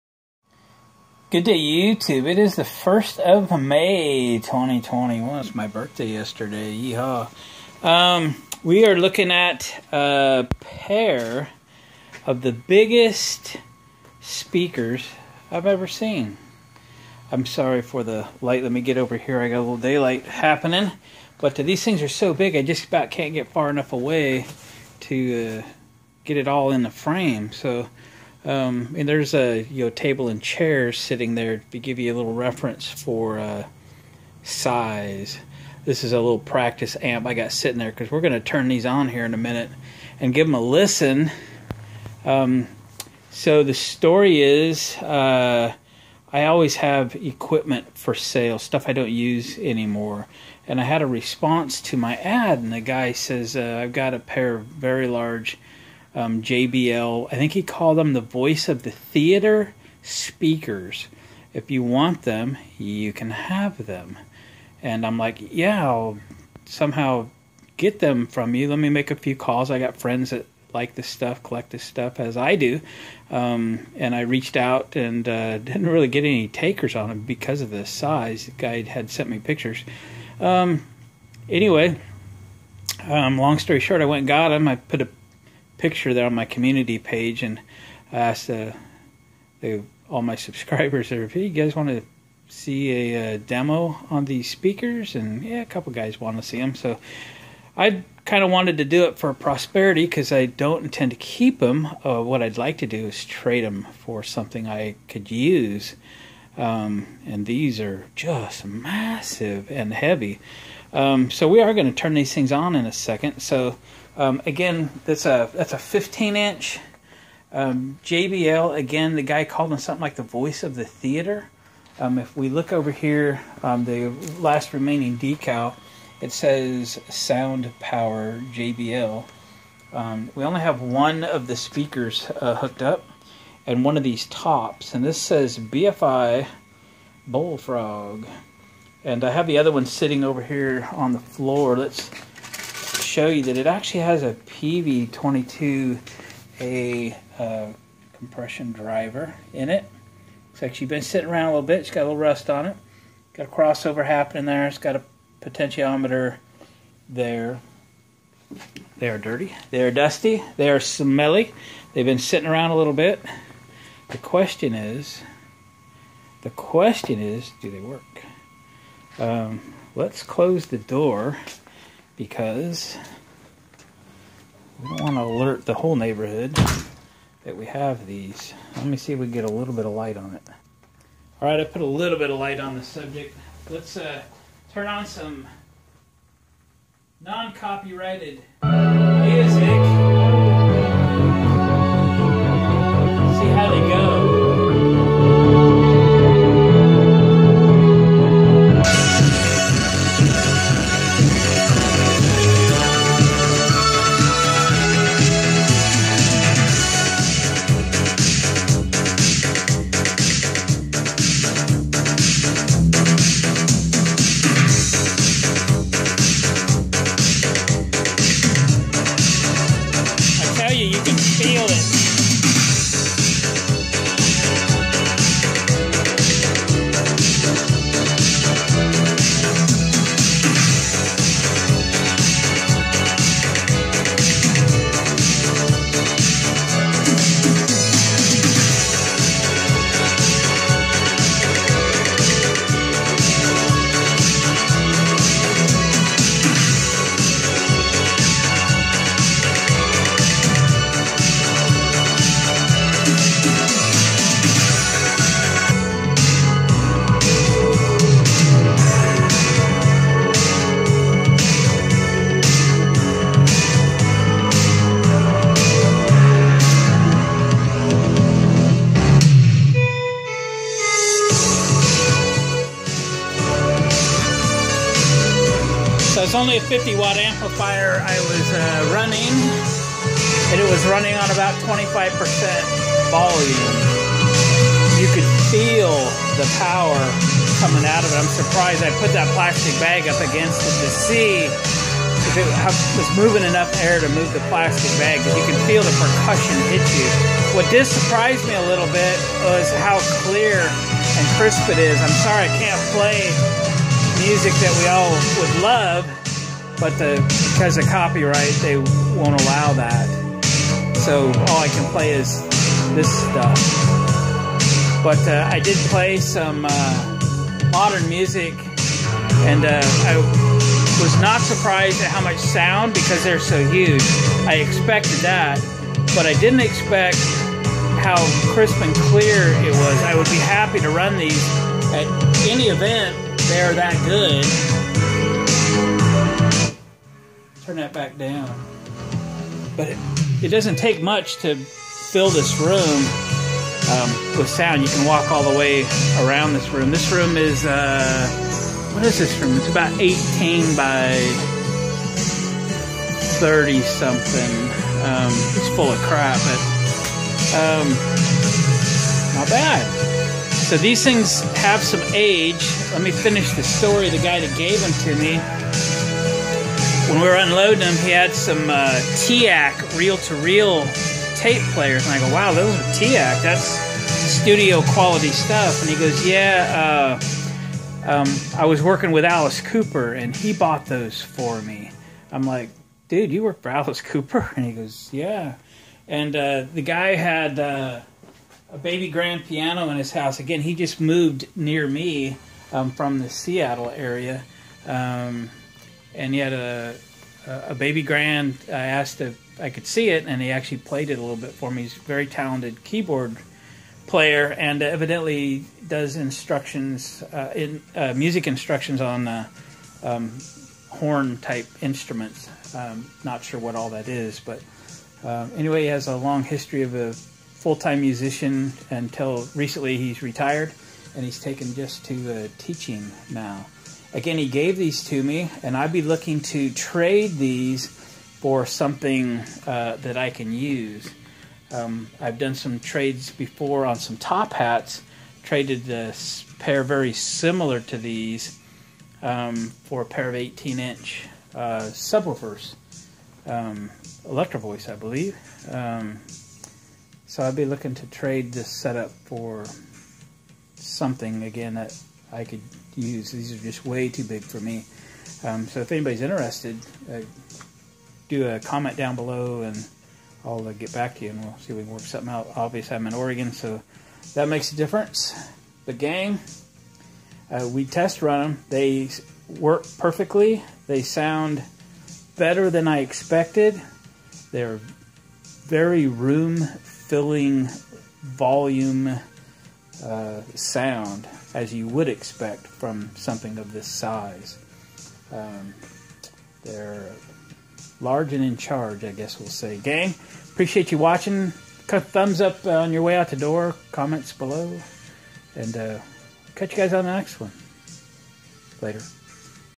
JBL Voice of the Theater Speakers. Let's hook these big boys up. Free Huge Speakers!
After filming this video, I played some good old Rick and roll on these bad boys and now I don't think I can let them go! I may have to restore them and keep them.